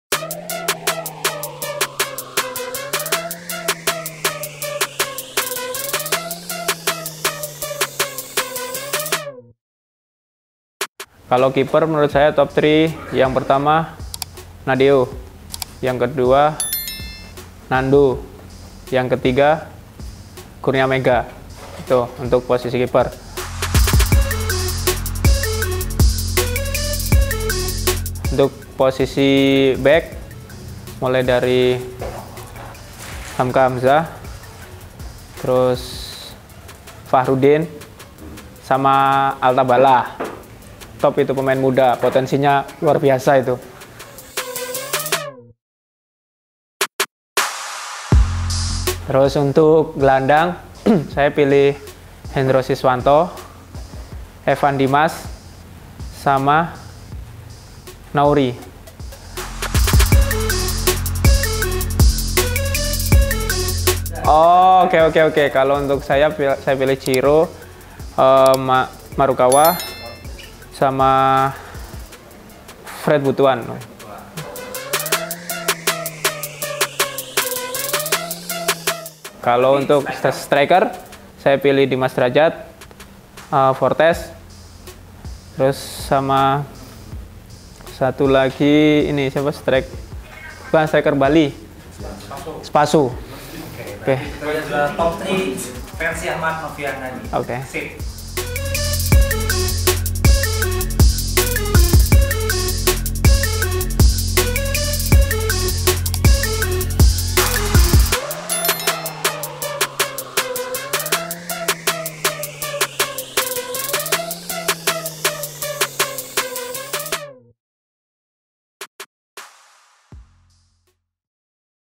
PEMAIN - PEMAIN TERBAIK LIGA INDONESIA VERSI AHMAD NOVIANDANI
Halo Sob! Kali ini winger Ahmad Noviandani akan memilih 3 pemain terbaik di Liga Indonesia pada setiap posisi dari kiper hingga striker.